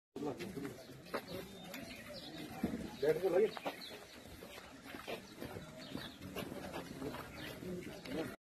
Five terrorists have been eliminated in Reban village of south Kashmir’s Shopian district.
Five terrorists have been eliminated by a joint team of JKP-SOG, IA-RR, &amp; CRPF in Reban village of south Kashmir’s Shopian district. Operation concludes with zero civilian casualties injury